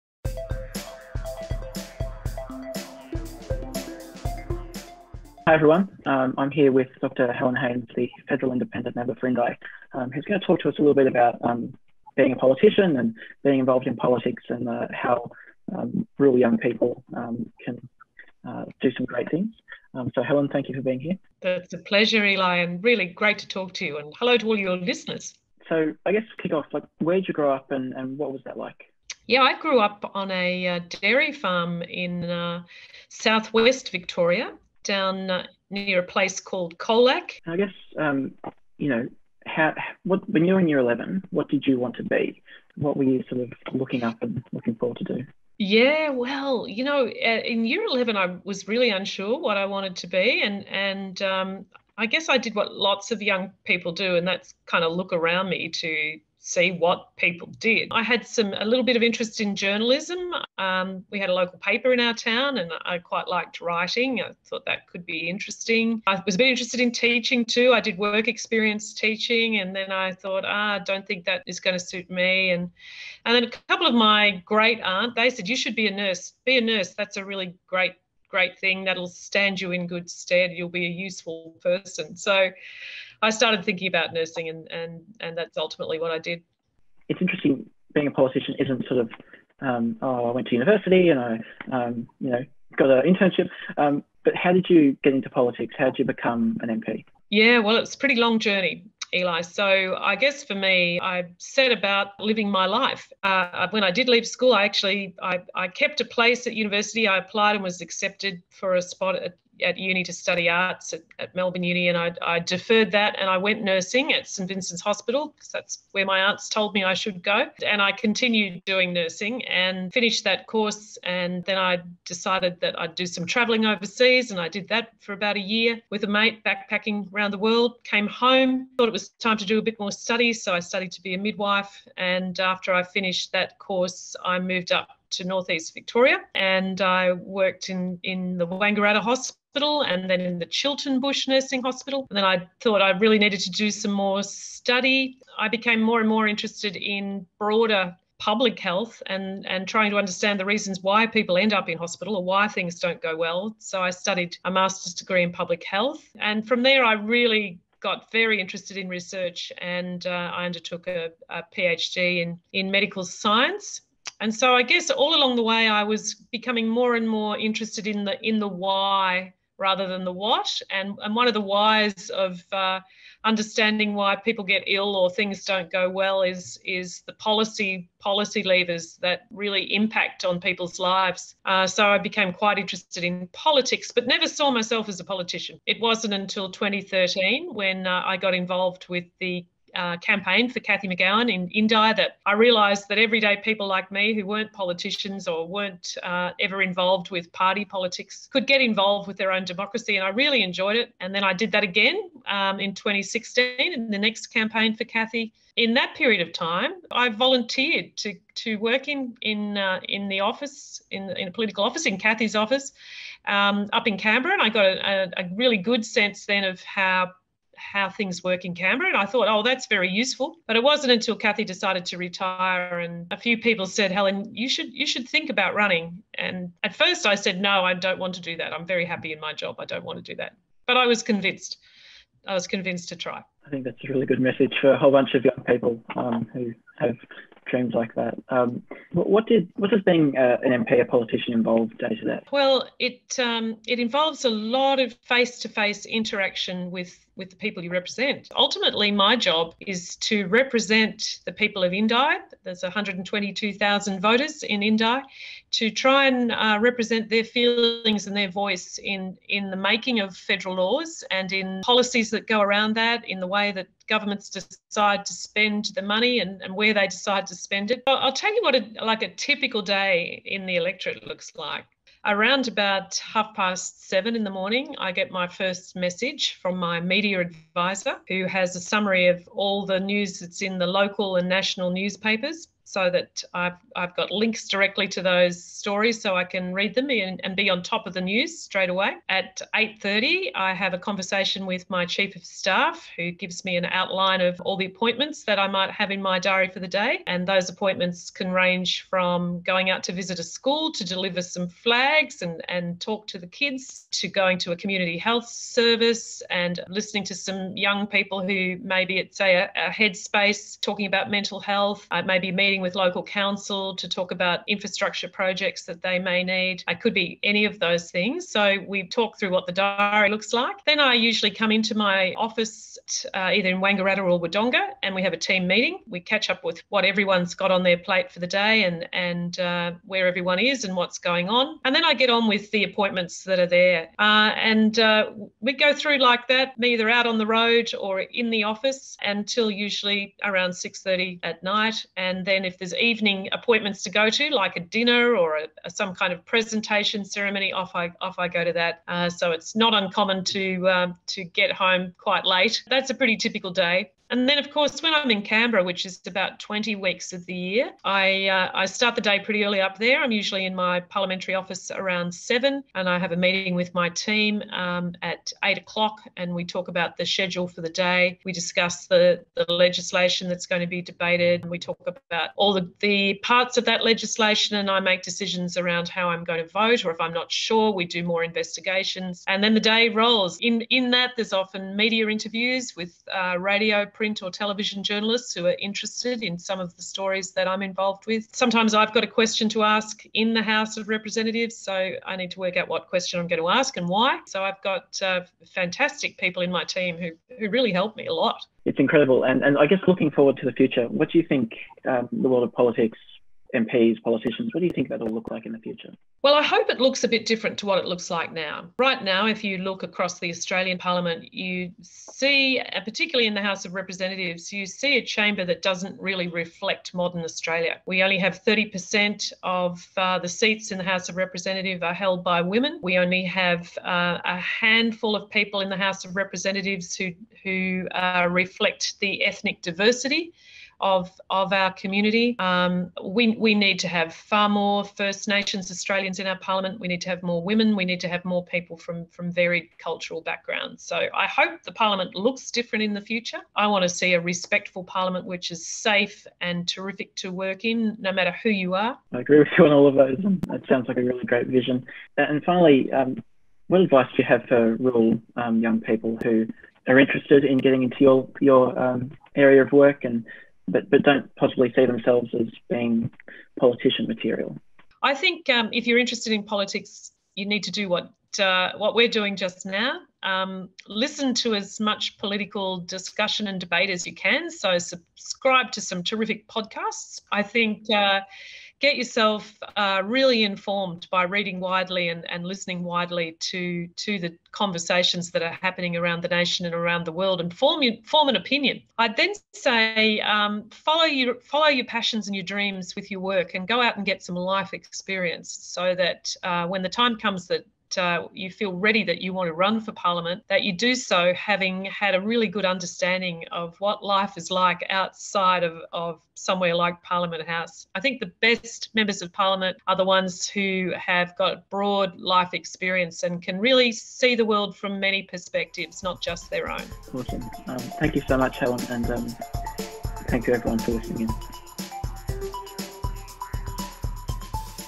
Rural Careers Series: Politics
Year 11 student Eli speaks to Dr Helen Haines, Federal Member of Parliament for Indi, about the journey that led her to politics. In year 11 Helen wasn't sure what she wanted to do in life, but on the advice of her great aunts began training to be a nurse. Eventually she became  more interested in research, policy and public health. Watch the interview for the rest of Helen's incredible journey to becoming a federal politician.